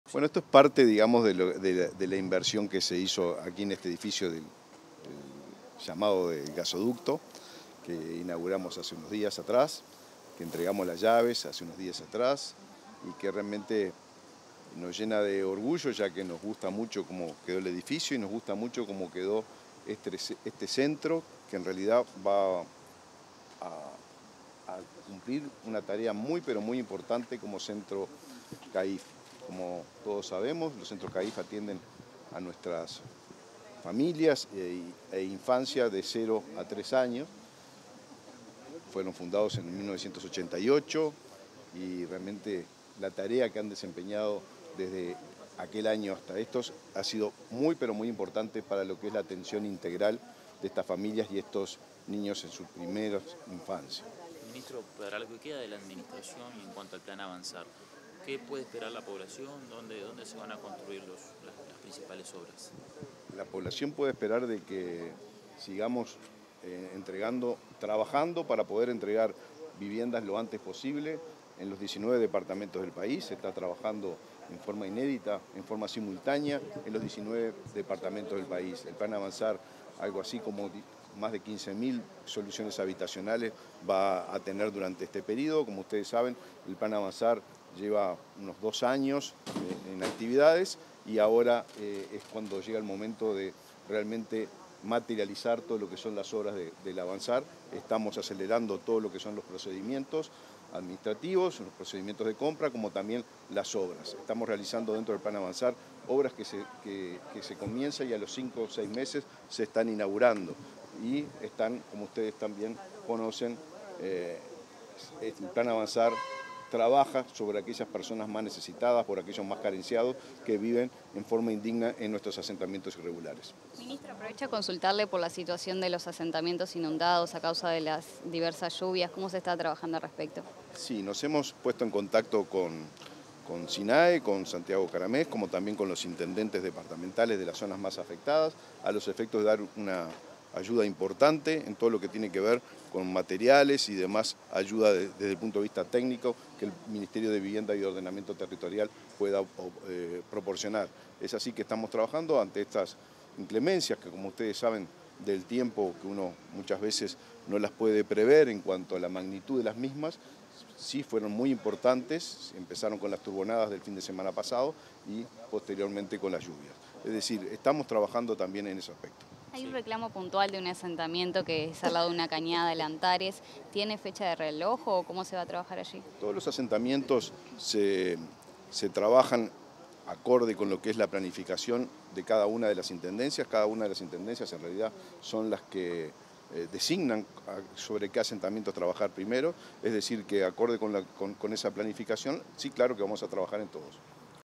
Declaraciones del ministro de Vivienda, Raúl Lozano.
Tras la firma de un comodato para instalar un centro de atención a la infancia y la familia (CAIF) en el barrio Sur de Montevideo, el ministro de Vivienda, Raúl Lozano, dialogó con la prensa.